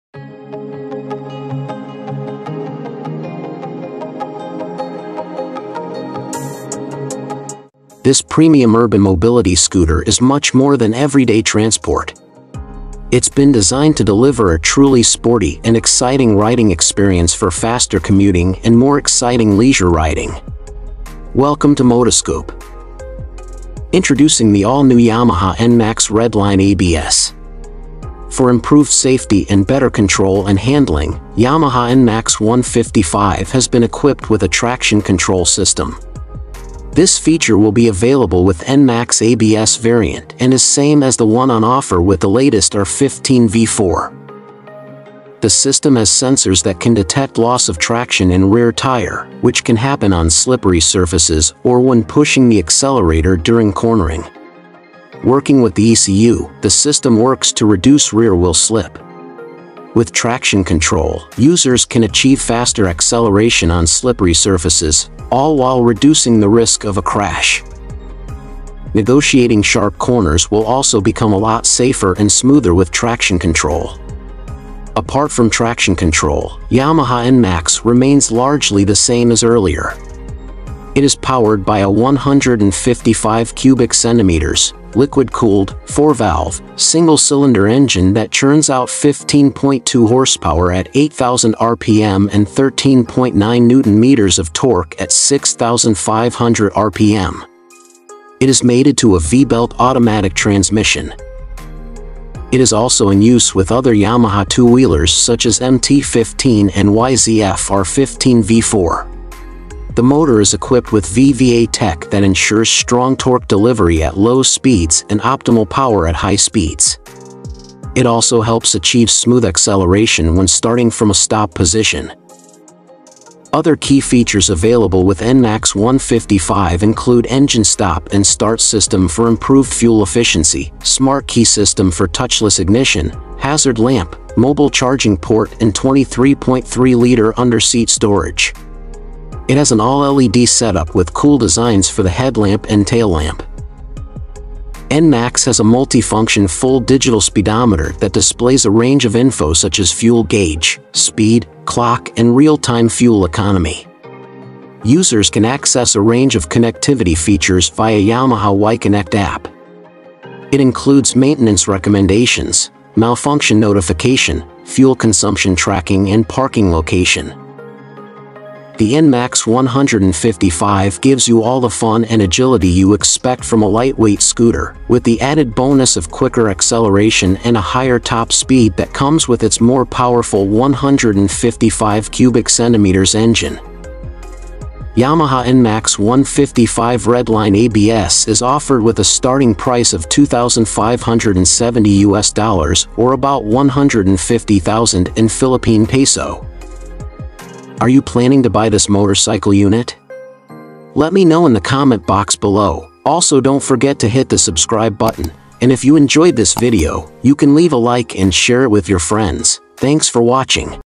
2022 Yamaha Scooter Gets its New Traction Control System Feature – NMAX ABS Redline Walkaround
2022 Yamaha Scooter Gets its New Traction Control System Feature – NMAX ABS Redline Walkaround